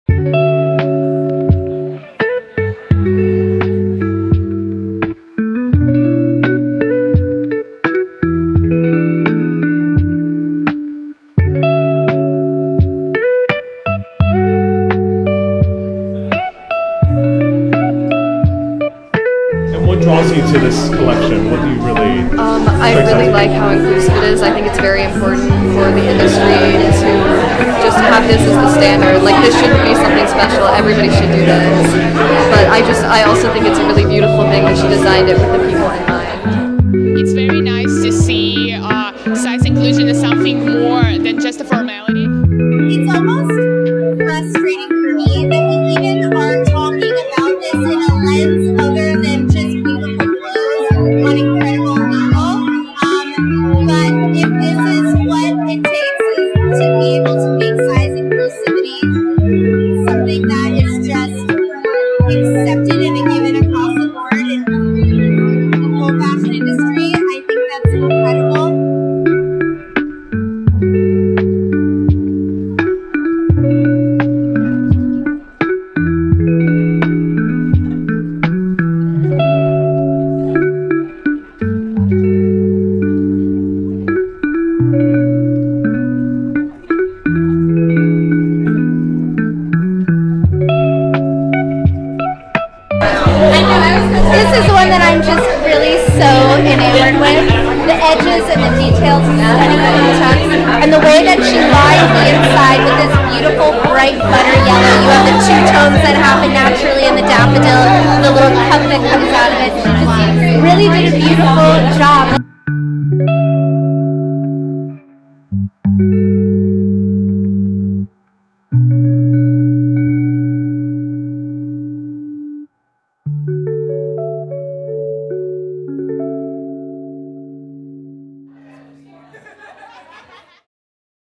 SmartGlamour: Couture For Everybody
Mallorie Dunn presents her couture capsule collection, November 1st 2024 in the Garment District, Manhattan. 10 models in 10 inclusive looks inspired by florals are presented in a gallery style setting. View and shop the pieces at smartglamour.com

Videographer: Ariel Viera

Models:
Tati Lewis
Patricia Birch
Alex Castro
Carine Calixte
Noctua Vindicta
Hillary Likos
Stephanie Mallick
Moet Cristal
Elah Seidel

Special thanks to:
Ian Campbell Dunn
Mia Jackson
Nicola Carravagio
Alexa Yam
Summer Kubota